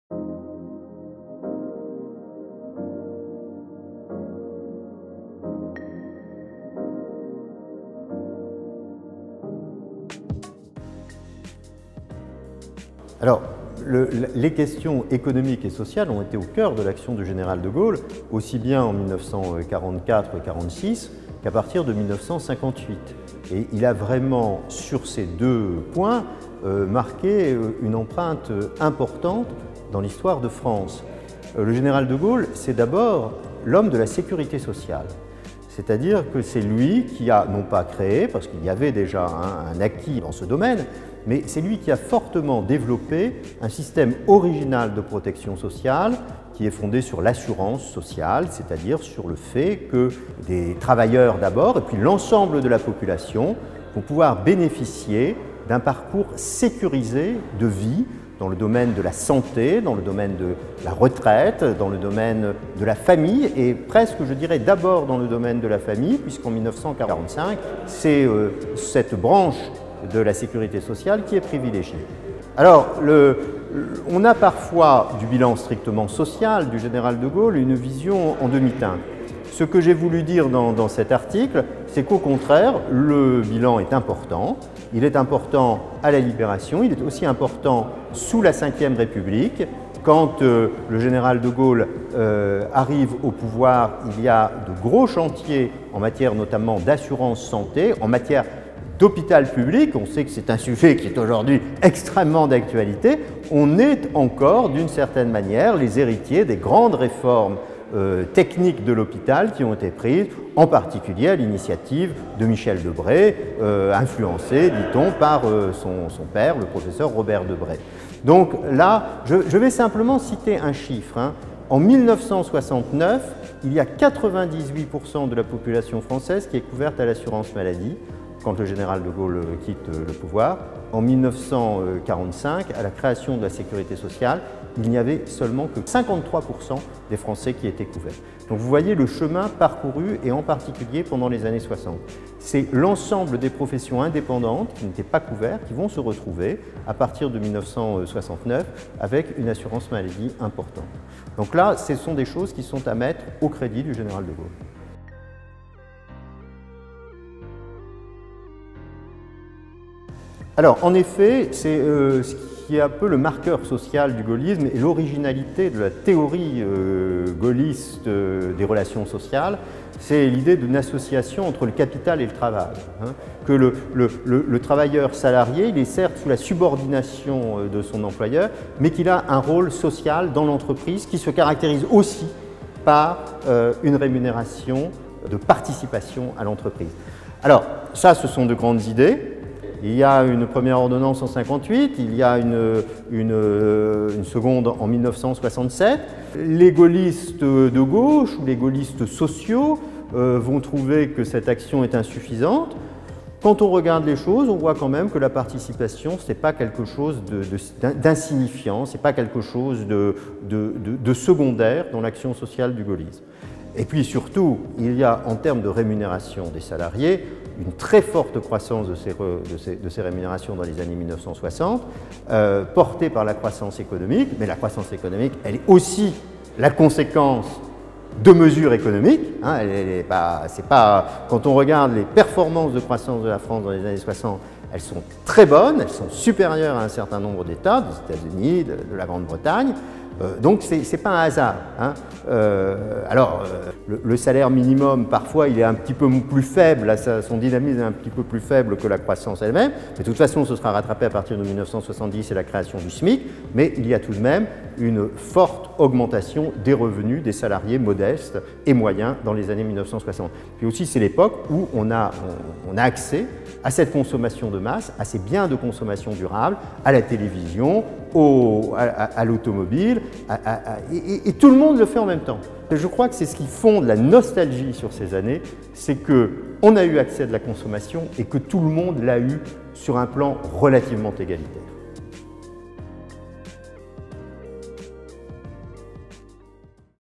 HEIP & Revue Politique et Parlementaire : interview de Frédéric Tristram sur le numéro De Gaulle
À l'occasion de la sortie du nouveau numéro de la Revue Politique et Parlementaire, intitulé "Bonjour De Gaulle, bonjour !", nous avons eu le plaisir d'accueillir Frédéric Tristram, Maître de conférences (HDR) en Histoire contemporaine à Paris I Panthéon-Sorbonne et professeur à HEIP. Découvrez son interview. 📽